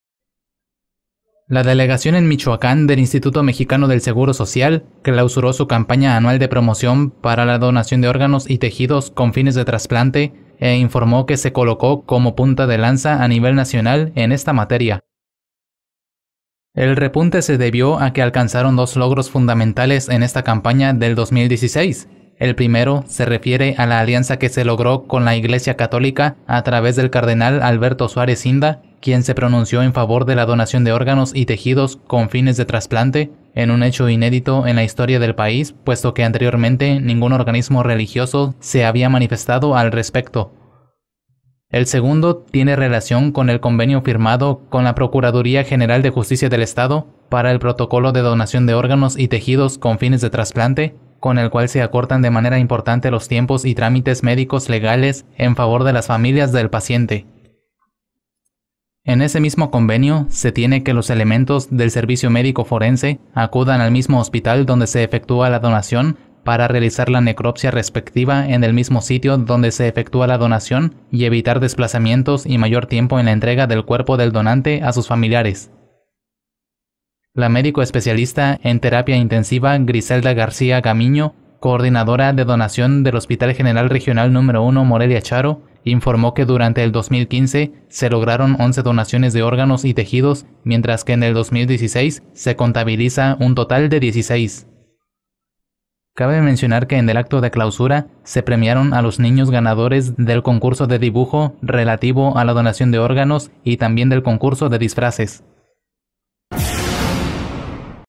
IMSS Michoacán se ubica como punta de lanza
*-La Delegación en Michoacán del Instituto Mexicano del Seguro Social (IMSS) clausuró su campaña anual de promoción para la donación de órganos y tejidos con fines de trasplante e informó que se colocó como punta de lanza a nivel nacional en esta materia.